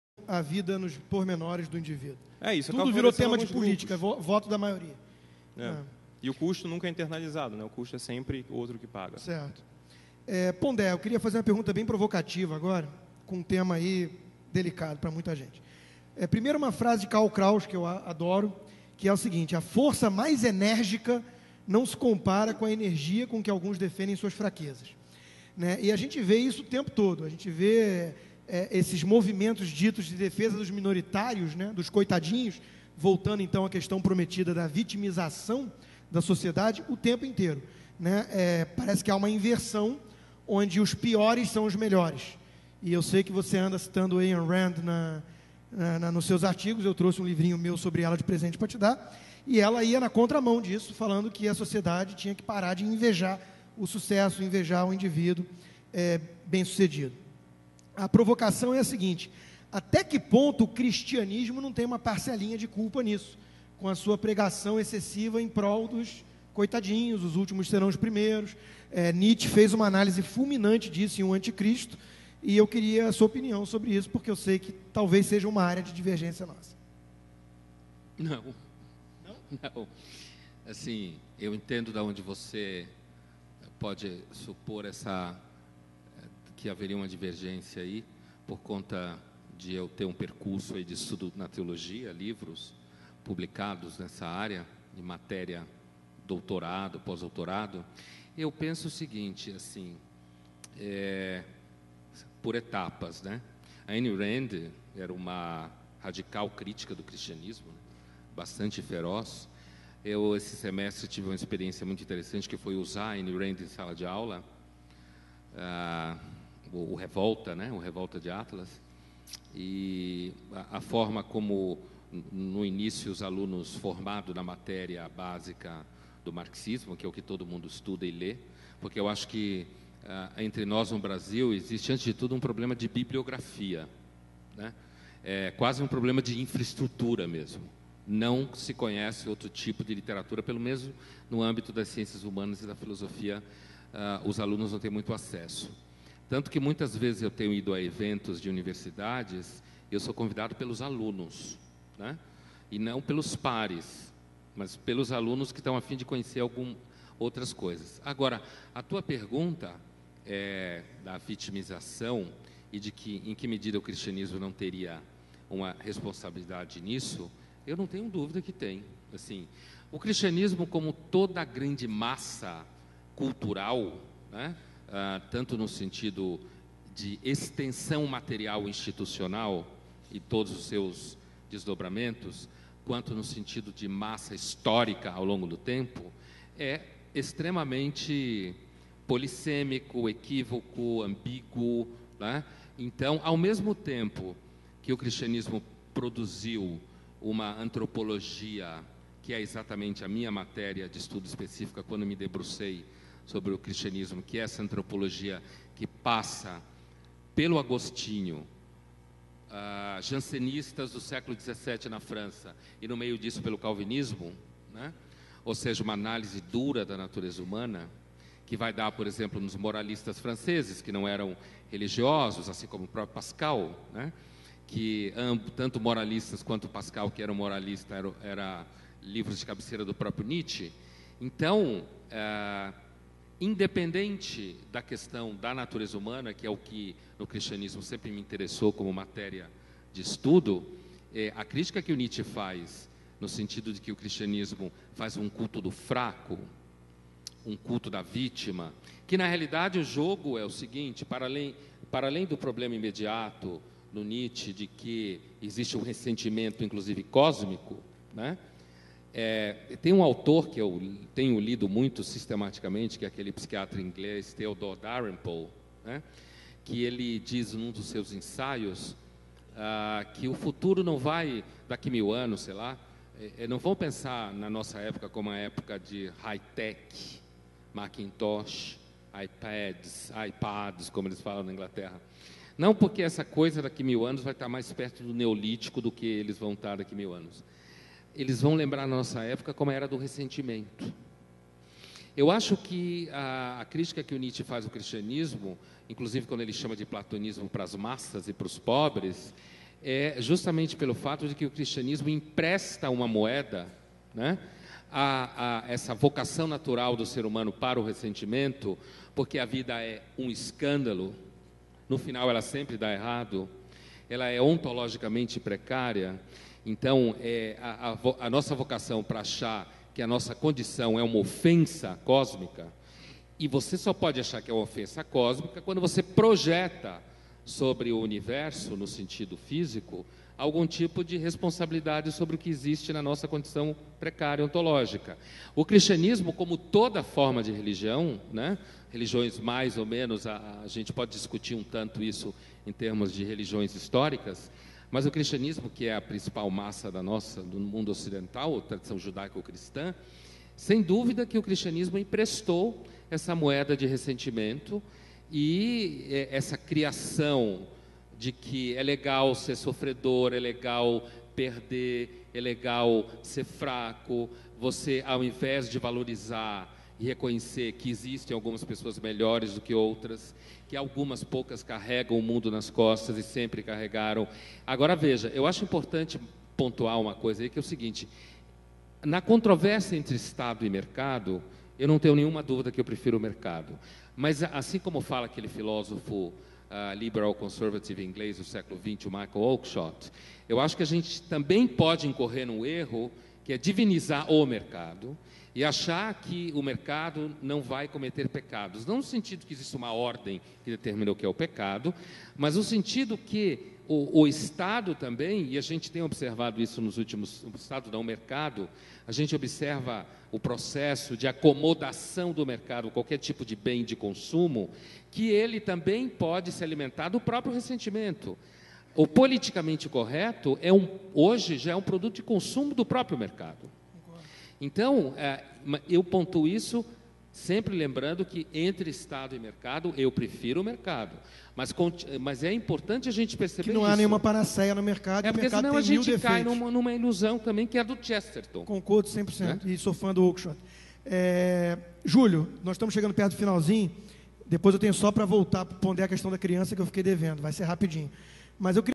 Pondé sobre o Cristianismo e a sociedade da vitimização
Trecho do Ideias em Movimento, onde pergunto a Pondé sobre a responsabilidade do Cristianismo na sociedade da vitimização, que enaltece os piores e ataca os melhores.